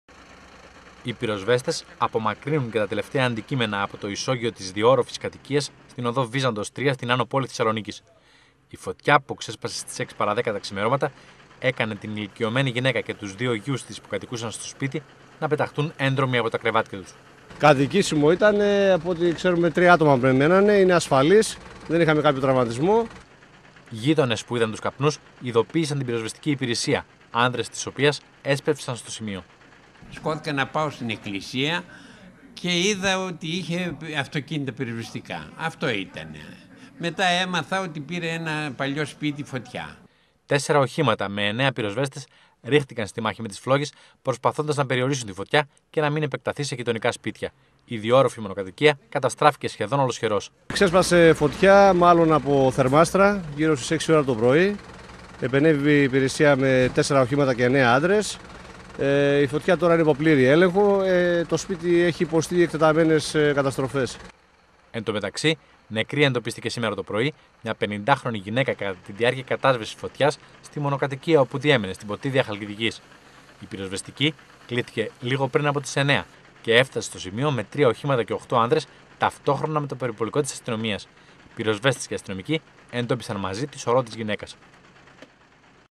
ΦΩΤΙΑ ΣΕ ΜΟΝΟΚΑΤΟΙΚΙΑ ΣΤΗΝ ΑΝΩ ΠΟΛΗ(TV100-180218)
Σχεδόν ολοκληρωτική καταστοφή σε μονοκατοικία της Άνω Πόλης προκάλεσε η φωτιά που ξέσπασε λίγο πριν από τις 6 τα ξημερώματα. Στο σπίτι κατοικεί μια ηλικιωμένη γυναίκα με τους δύο γιους της, οι οποίοι πρόλαβαν να βγουν πριν οι φλόγες επεκταθούν. Αιτία της φωτιάς ήταν μία διαρροή στη θερμάστρα πετρελαίου. Στην Ποτίδαια Χαλκδικής μια 50χρονη γυναίκα εντοπίστηκε νεκρή κατά τη διάρκεια κατάσβεσης φωτιάς στη μονοκατοικία όπου διέμενε.
Ρεπορτάζ: Παναγιώτης Μανής
Κάμερα: Άκης Σπορίδης
Μοντάζ: Όλγα Τσιακίρη
Μιλούν: Ελεθέριος Φάνης(πυρονόμος),
(κάτοικος περιοχής)